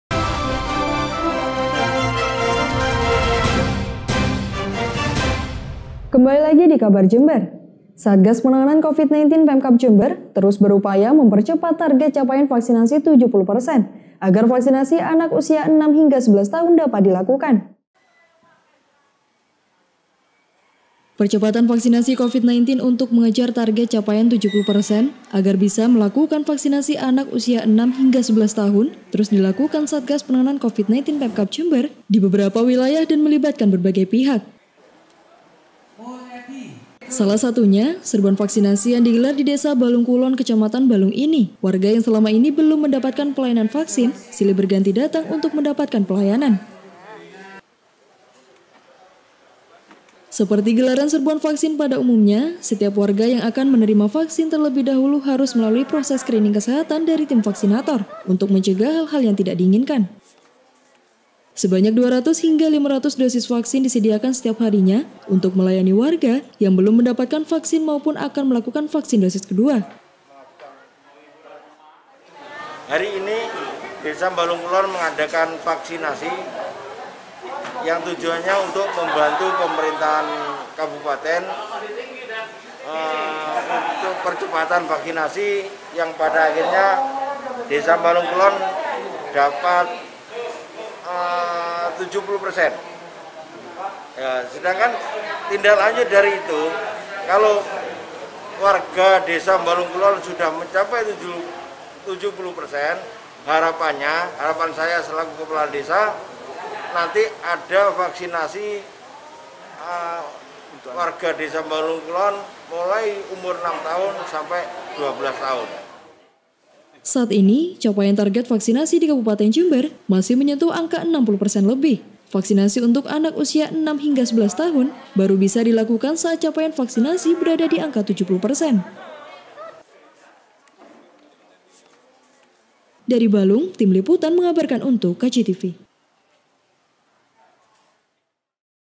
PERCEPATAN VAKSINASI COVID-19 DI DESA BALUNG KULON | KABAR JEMBER
BALUNG, KJTV JEMBER - Satgas penanganan Covid-19 Pemkab Jember, terus berupaya mempercepat target capaian vaksinasi 70 persen, agar vaksinasi anak usia 6 hingga 11 tahun dapat dilakukan.